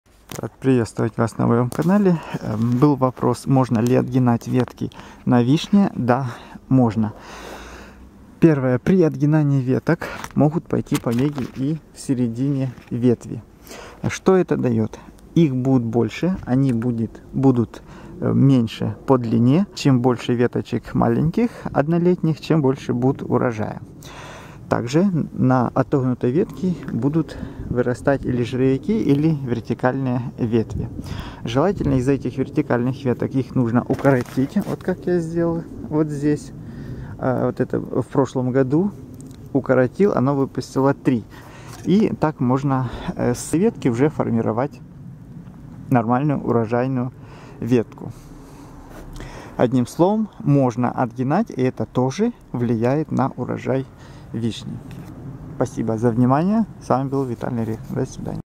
Отгибание ветвей на вишне. Вопрос - Ответ.
Отгибание ветвей на вишне возможно в любое время года.
больший эффект даёт отгибание однолетних побегов от 70см длины(если такие есть).